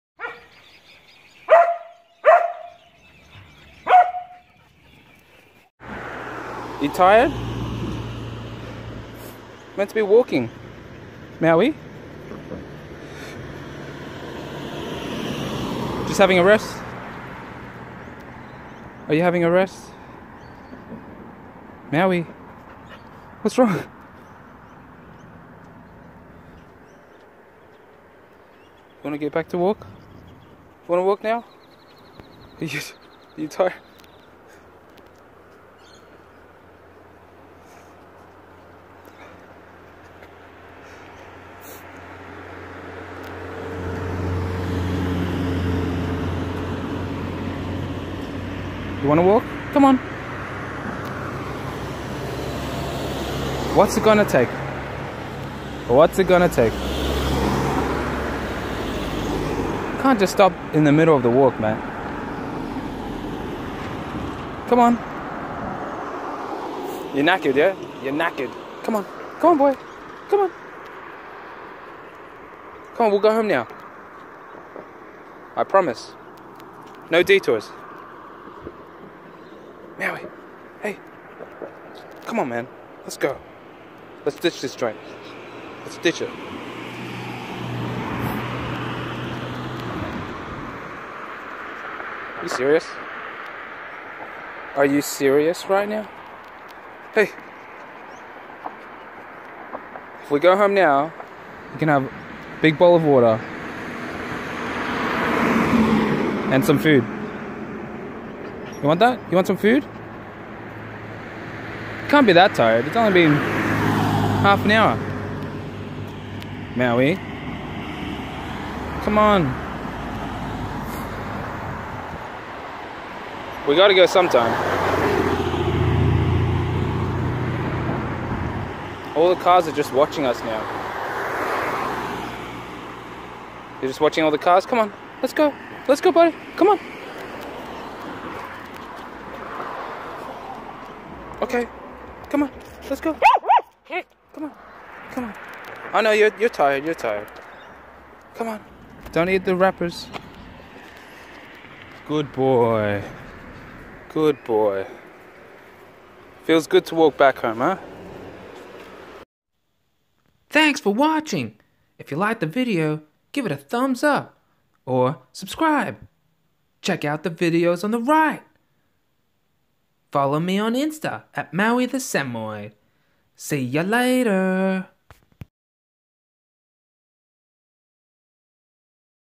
My Dog gave up halfway through his walk
My dog gave up halfway through his walk. A funny video of Maui the samoyed refusing to continue walking during his afternoon walk. Daddy tries to bribe him with incentives to continue walking.

You can follow maui on instagram @mauithesamoyed or check out his other videos on this channel